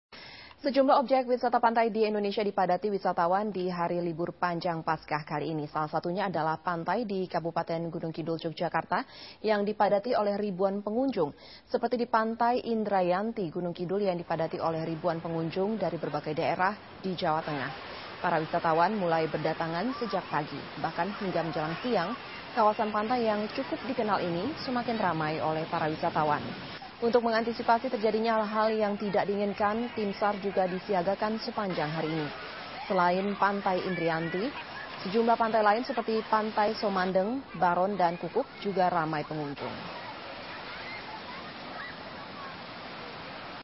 SAR Bersiaga Amankan Pengunjung Pantai Indrayanti
Wonosari: Pantai Indrayanti di Gunungkidul, DI Yogyakarta, menjadi tujuan wisata di hari libur panjang bertepatan dengan perayaan Paskah 2013. Tim SAR pun bersiaga menjaga keamanan dan kenyamanan para pengunjung, Sabtu (30/3).

Pengunjung dari berbagai daerah mendatangi Pantai Indyarati sejak pagi. Selain itu, pengunjung mendatangi Pantai Somandeng, Baron, dan Kukup.